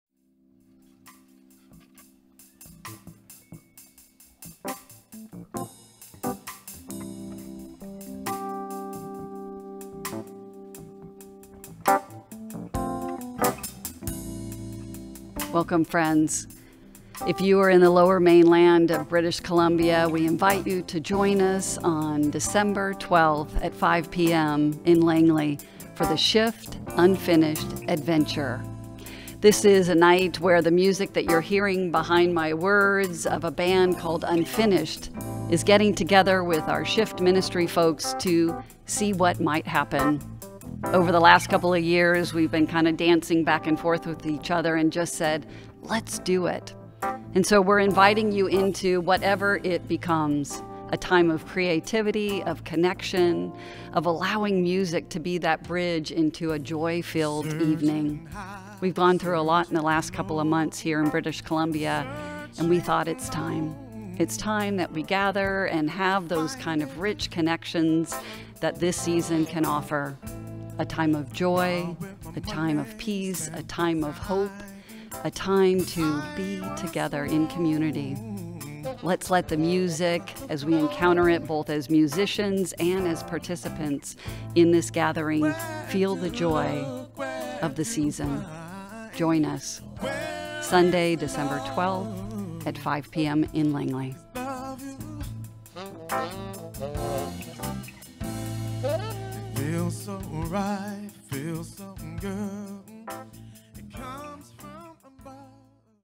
Shift UnFinished ADVENTure
Join us on Sunday, December 12, 2021, 5pm
SHIFT! with the Band Unfinished
Hosted by Sophia Ducey, Neerava Kroeger and Brad Jarvis

IN PERSON at United Churches of  Langley
21562 Old Yale Road, Langley, BC V3A4M8

for an evening of music and conversation
as we celebrate JOY during the 3rd week of Advent
with special guest band: Unfinished

Feel the intertwining of Joy that gives birth to Creativity
Let's create a JOY-filled evening together.